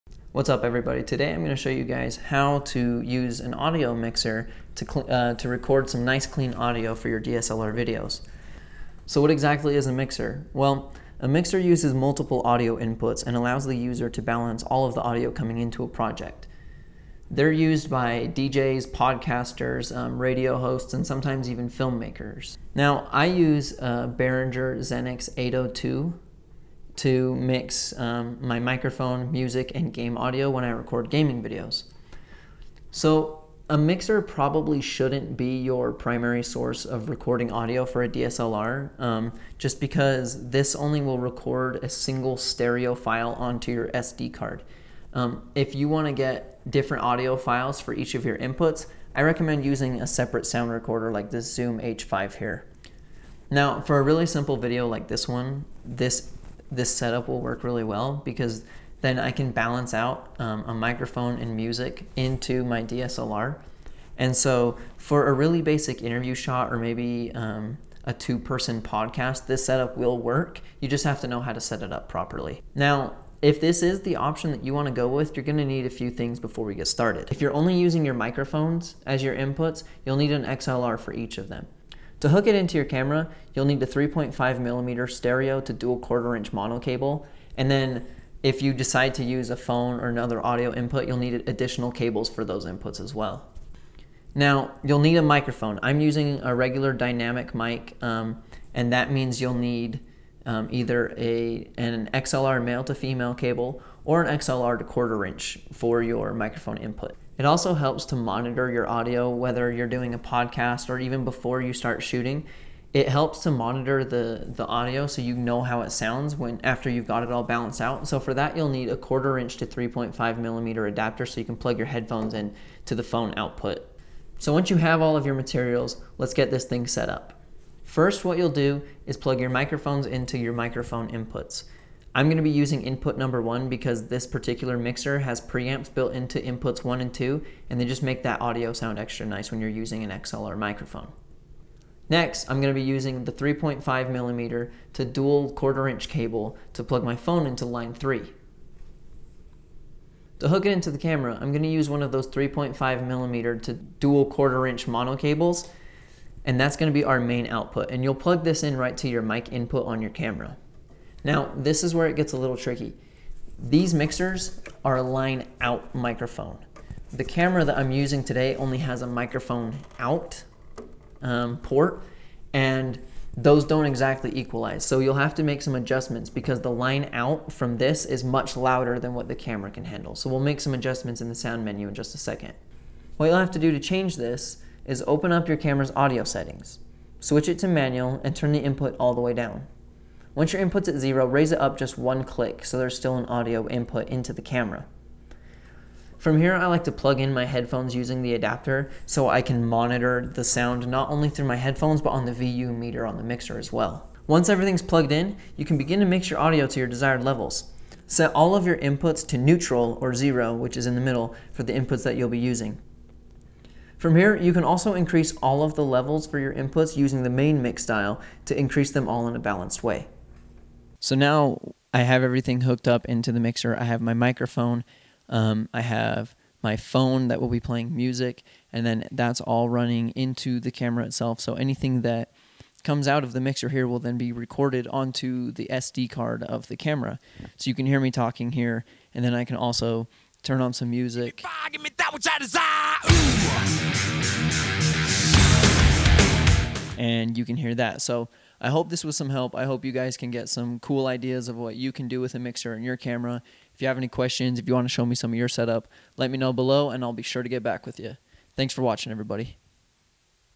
Recording Tips- How to Use an Audio Mixer with a DSLR Camera
Today I show you how to use an audio mixer with your DSLR camera. Using a mixer is a great way to record clean audio as well as mixing multiple inputs with your video.